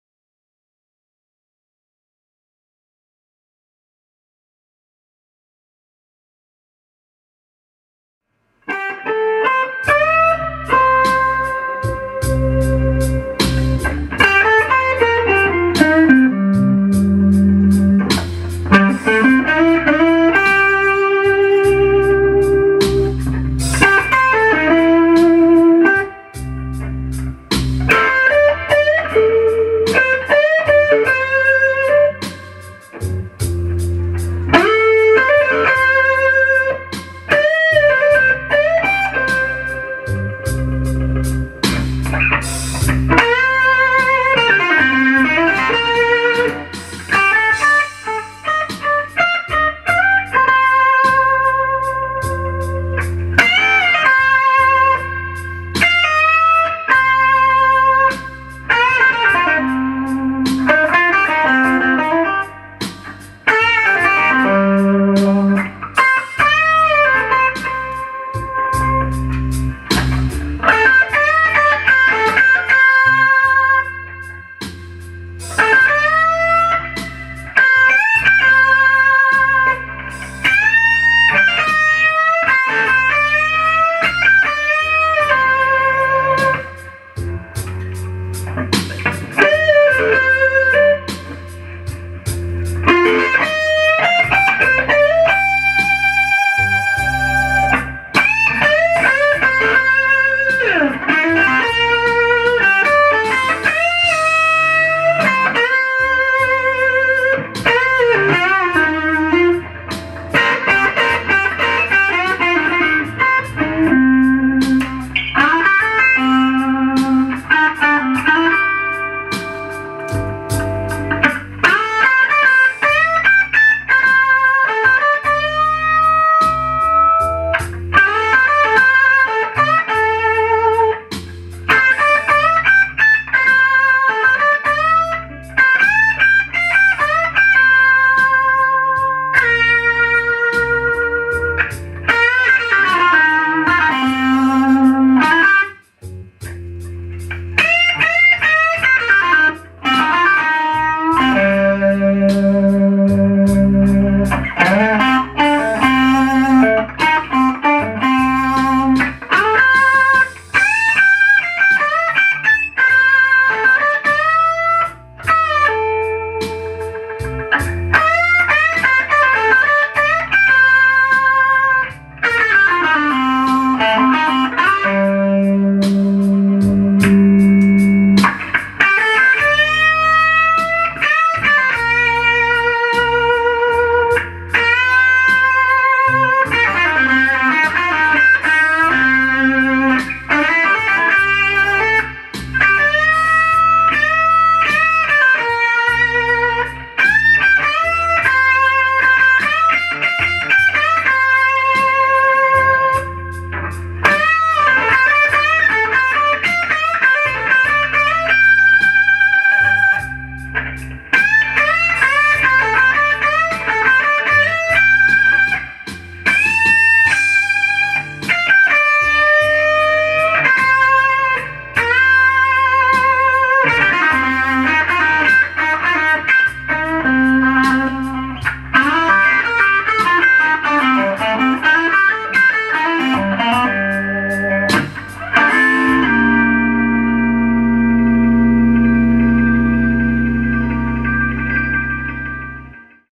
Corsa Guitars, MAN-011, all pickup positions
Corsa Guitars Limited Production MANALISHI Model, MAN-011, Cherry Burst, 6 lbs., 10 oz. (3.01Kg.). Upgraded over LCPG model, with select very lightweight honduran mahogany one piece back and neck, Brazilian rosewood fingerboard, aged fret markers, highly flamed maple top, NTN Aged finish with high gloss top, holly headstock overlay, celluloid pearl inlaid headstock graphics, Faber® HYBRIDge™ locking bridge, Lifton case by Faber-Germany.